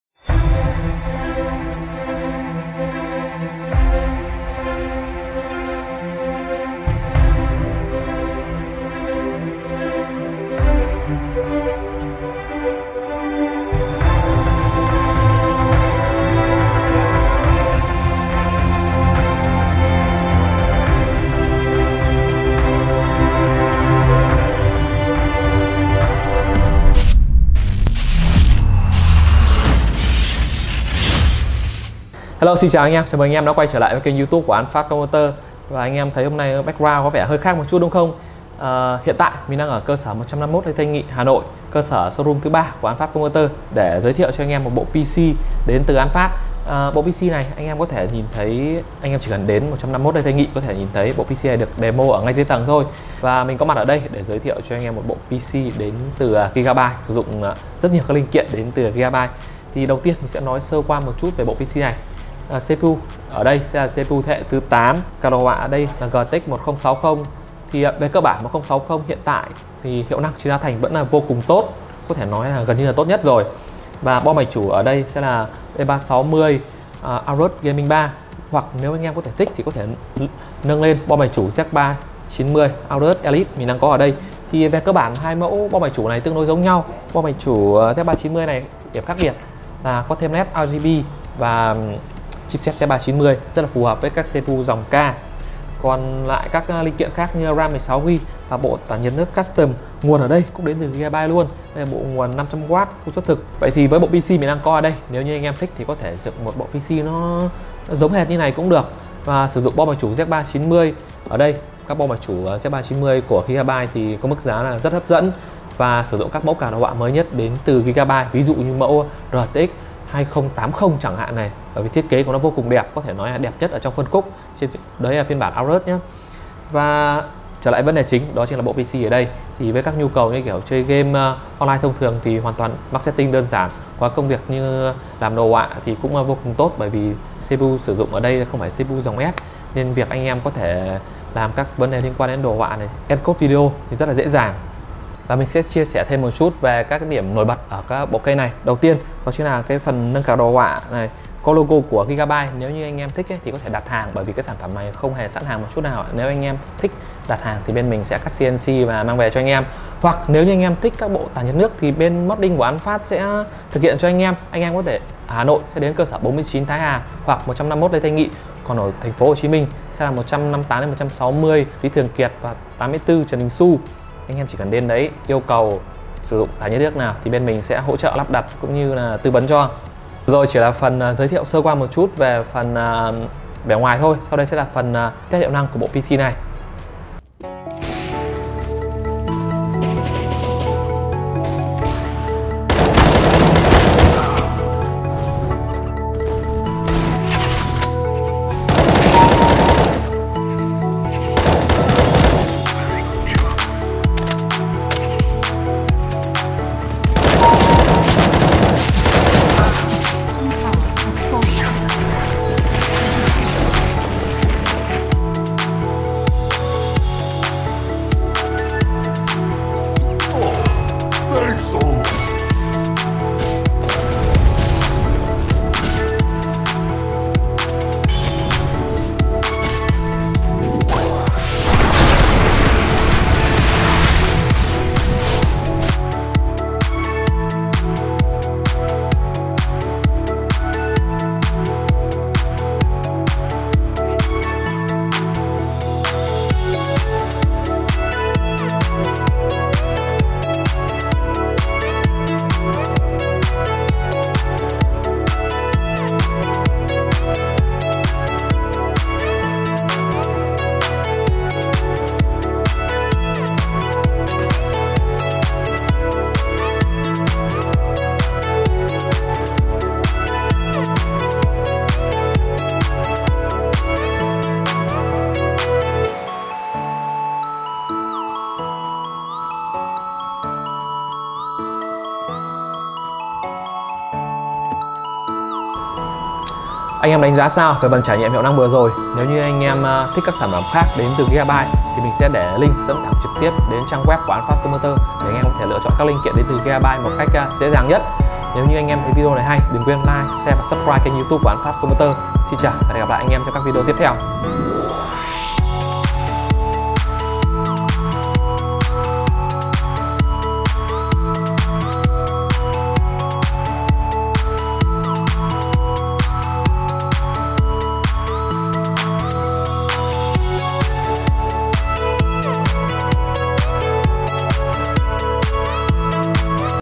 Siêu phẩm đến từ GIGABYTE có gì HOT????| An Phat PC
👉Xin chào các bạn ngày hôm nay mình sẽ giới thiệu cho các bạn bộ PC cực đẹp với rất nhiều các linh kiện đến từ Gigabyte .
👉Bộ PC này được trưng bày ở ngay showroom 151 Lê Thanh Nghị - Hai Bà Trưng - Hà Nội.

📌Và đừng quên anh em có các điều gì thắc mắc đừng quên để lại COMMENT ở phía dưới nhé.
SIÊU THỊ MÁY TÍNH AN PHÁT